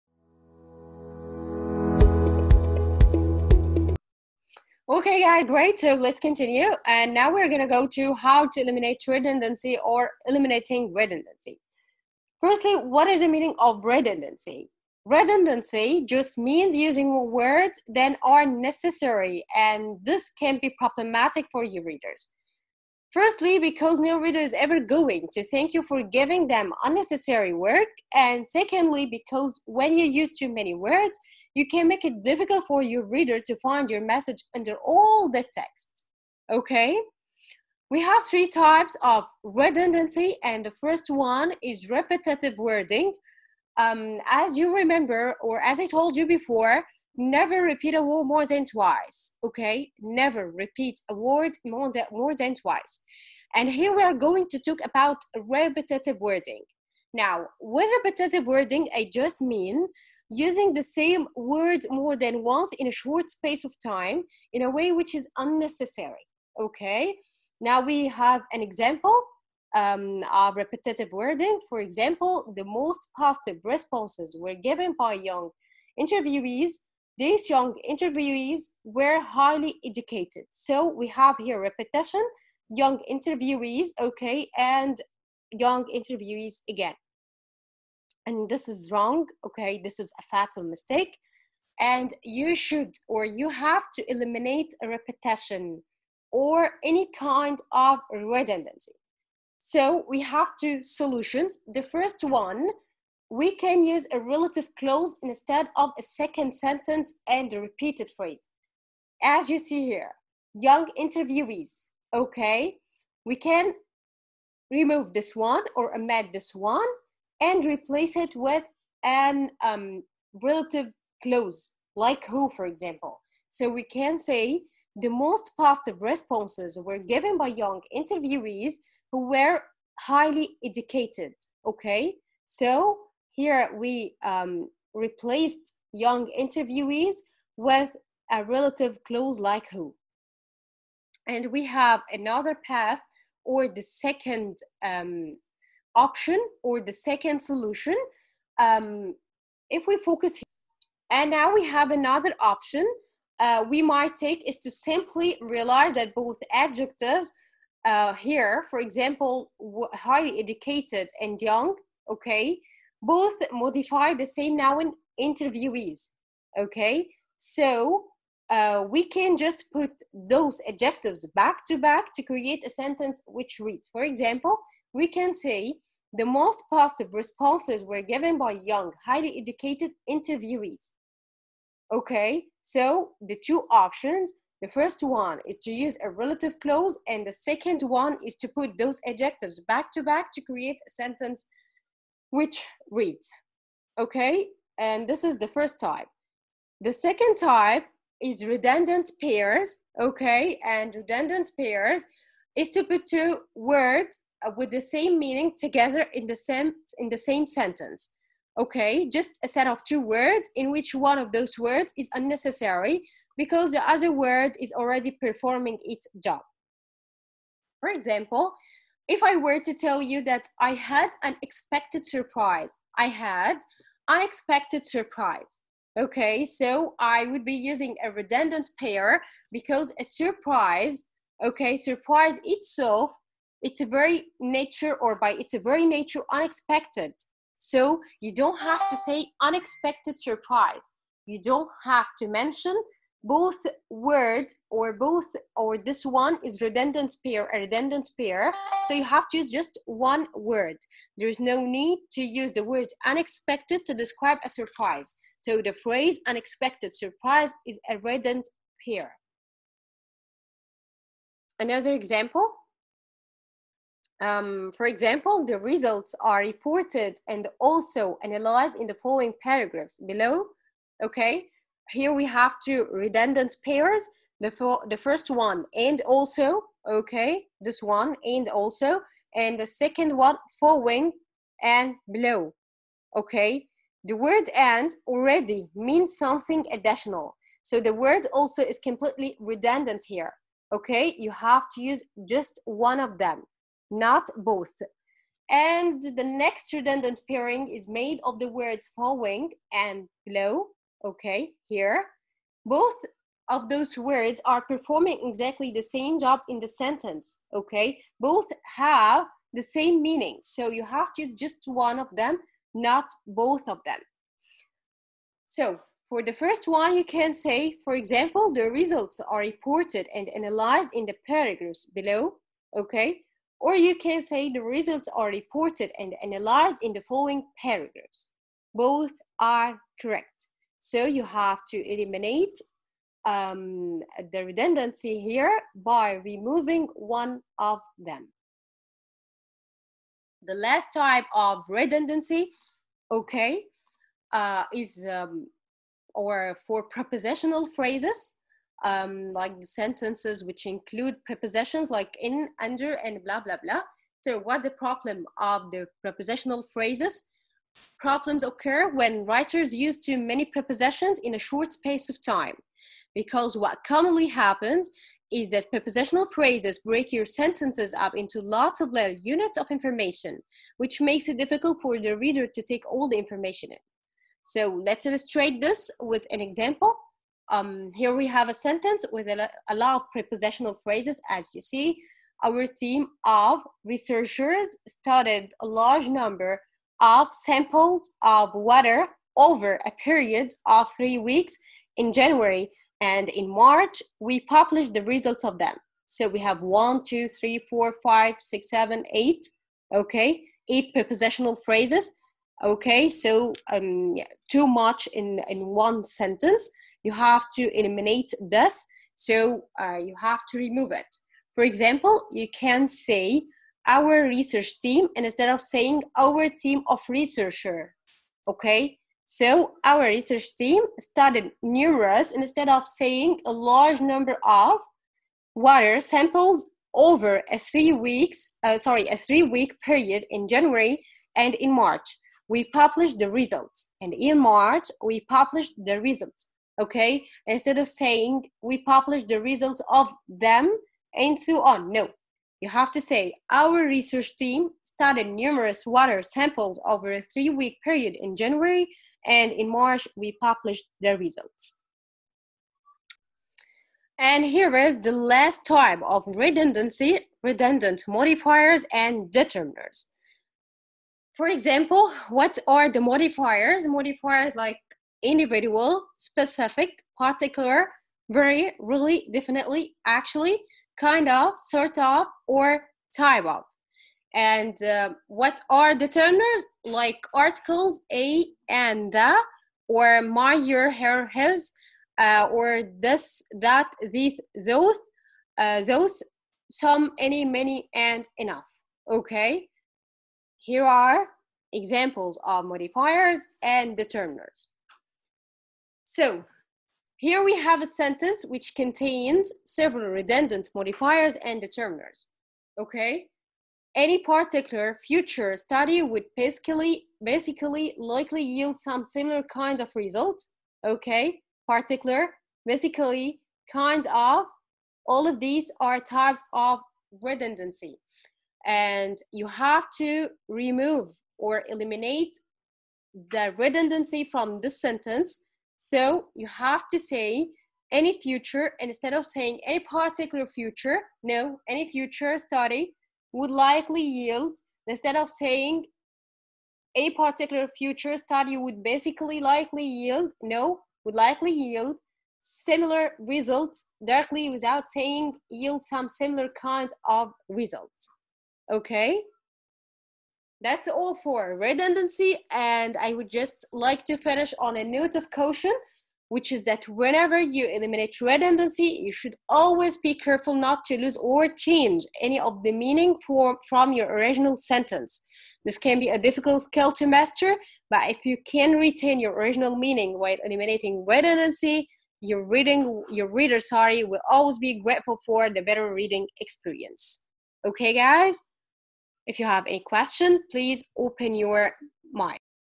IELTS: Eliminating Redundancy
IELTS: Eliminating Redundancy: A sentence contains a redundancy when it has two or more words, phrases or clauses expressing the same idea. As only one of these words or phrases is required, the other(s) should be removed. Here are a few tips to eliminate redundancy from your writing.